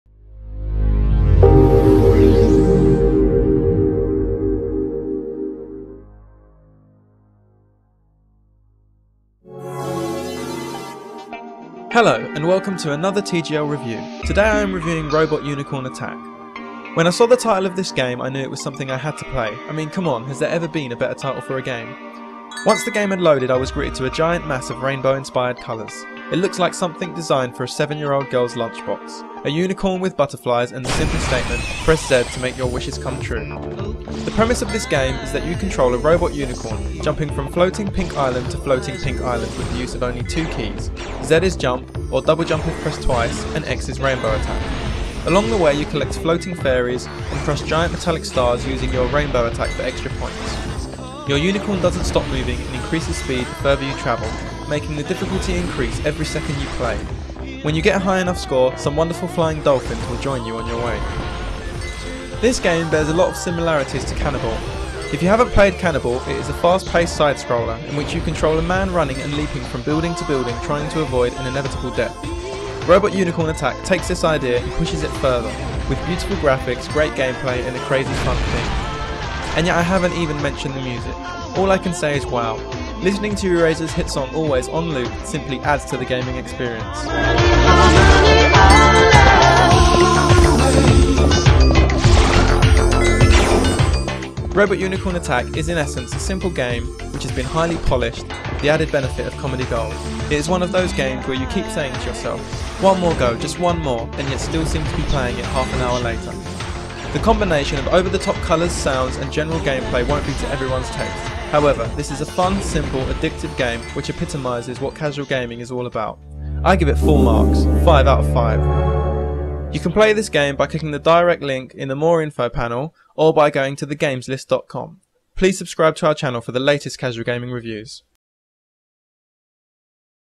Robot Unicorn Attack Flash Game Review
presents a review for the flash game "Robot Unicorn Attack".